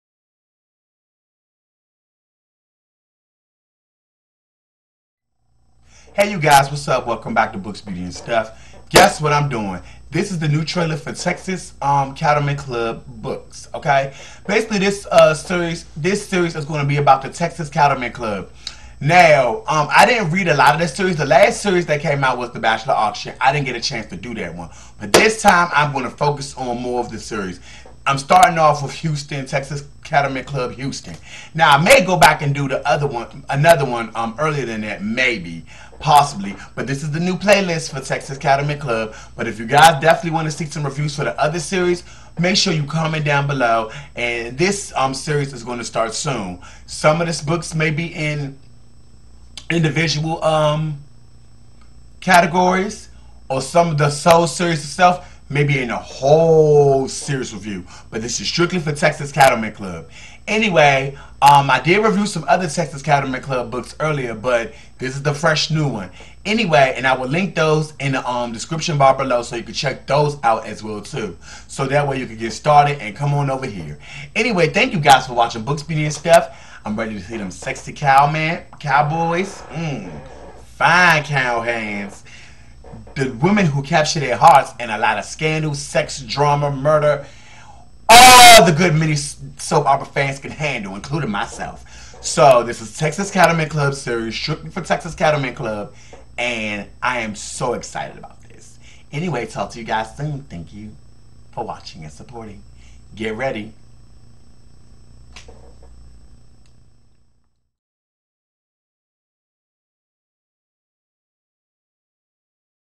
Texas Cattleman's Club: Trailer
Twenty years ago HarlequinBooks released a series that definitely changed the game in romance and soap operas: Texas Cattleman's Club series. Whew! Two decades of sex, drama, love, marriage, babies, fights, murders, envy, sexy foremen, beautiful women, hot ranch hands in a nice page turning book or books.

As we hit the 20th year anniversary, It seemed aproppriate for me to start a playlist exclusively to and for the groundbreaking series.